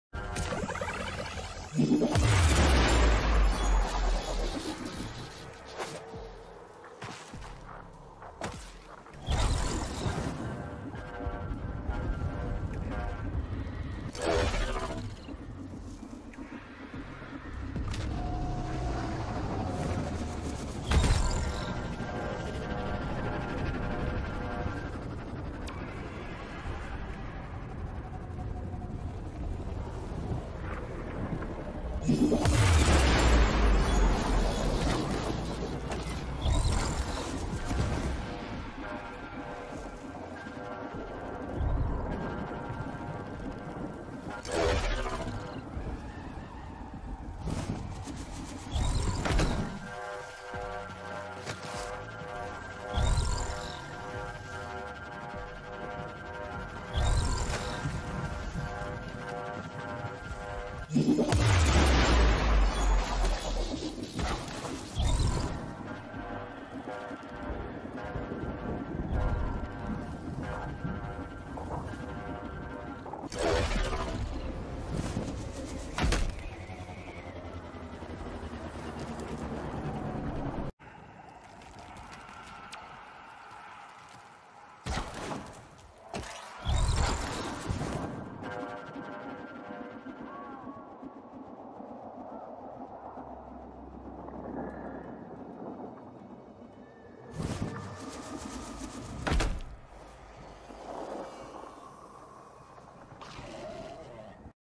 Don't fly too close to the sun!
your wax wings will melt.

400m roughly.

spoiler alert, I don't show my build.

I can still get further. when I make that video, I'll post the build.